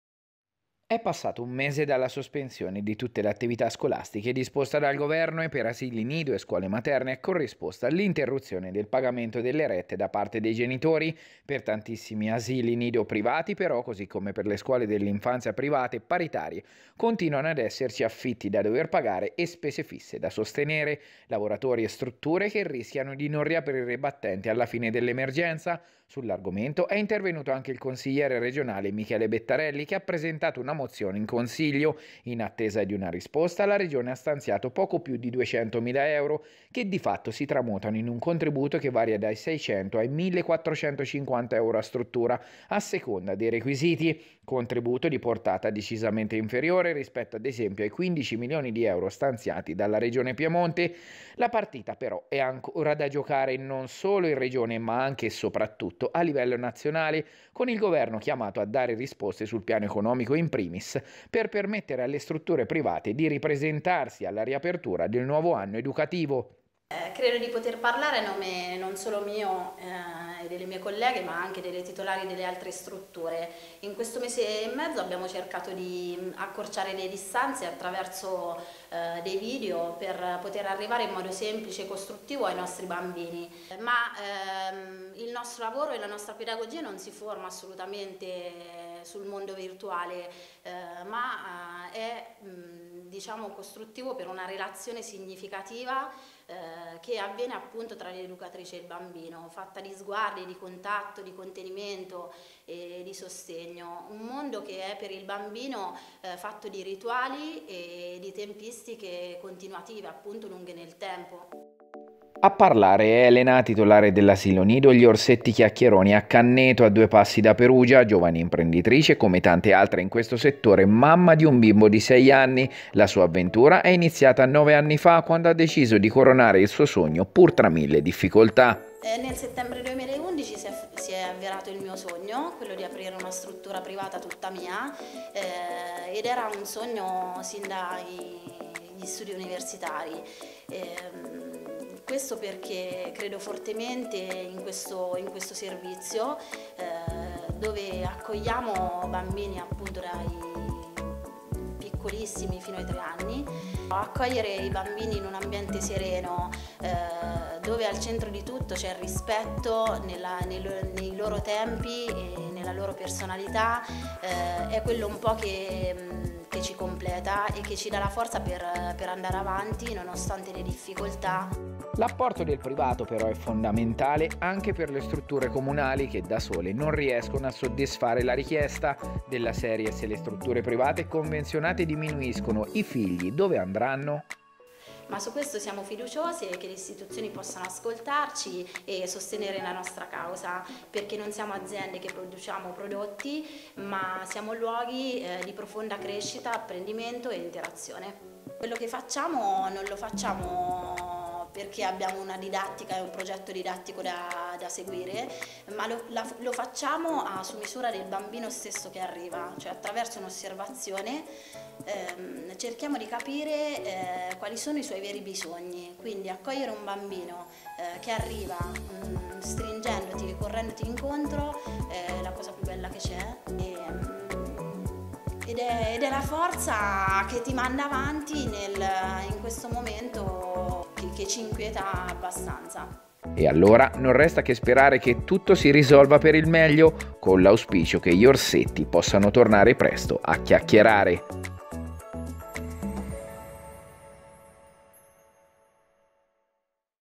I NIDI PRIVATI UMBRI LANCIANO L’ALLARME: SI RISCHIA LA CHIUSURA
Le strutture private educative umbre lanciano l’allarme. Per ora la regione ha stanziato in contributo che va da 600 a 1450 euro a struttura. Il racconto di Elena, titolare dell’asilo “Gli Orsetti Chiacchieroni” a Canneto.
Servizio di Nicola Agostini